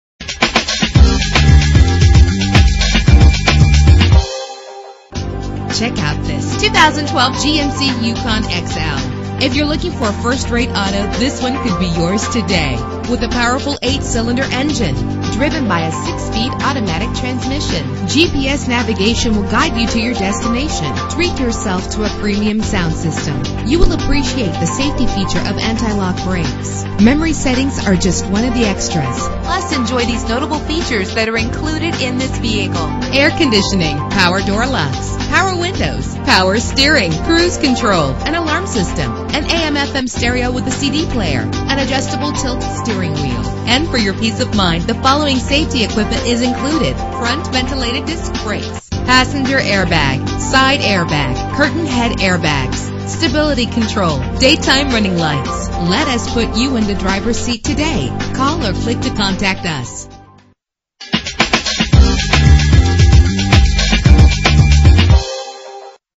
2012 GMC Yukon XL - Muskegon MI
Year: 2012
Make: GMC
Model: Yukon XL
Trim: 1500 Denali
Engine: 6.2 liter 8 cylinder 16 valve
Transmission: 6-Speed Automatic
Color: Quicksilver Metallic
Mileage: 2
Address:
2474 Henry St.